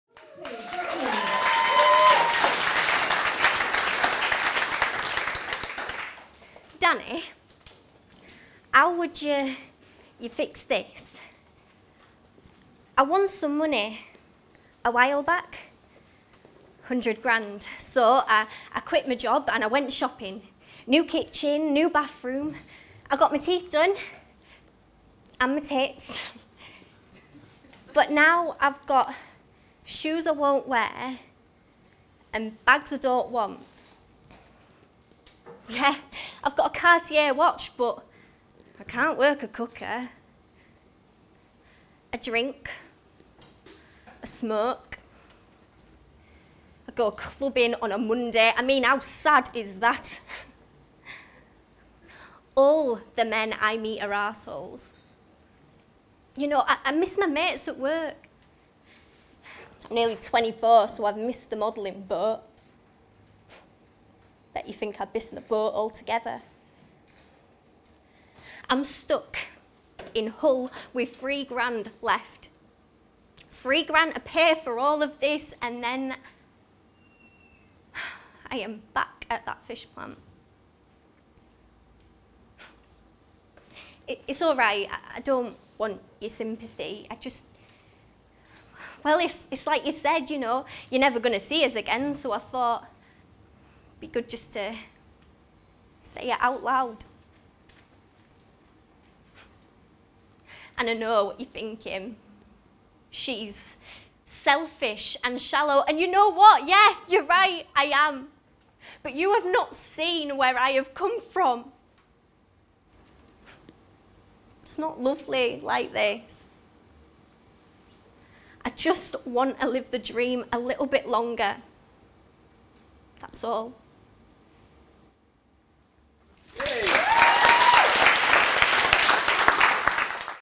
Cerys Evans | Ladies Down Under - Monojam 2019
Cerys Evans performs “Ladies Down Under” written by Amanda Whittington for Monojam 2019, held at the Antenna Media Centre in Nottingham.

Monojam is a showcase event organised by The Actors Workshop Nottingham and the Nexus Cast & Crew Network - East Midlands.  The night saw 25 actors perform in front of a full audience and a top industry judging panel. Past events have seen actors win paid acting roles, land agents and more.

Thanks to everyone behind the making of this video!

PLEASE NOTE: 'Monojam' in the new name for the actors showcase event formerly known as 'Monologue Jam'.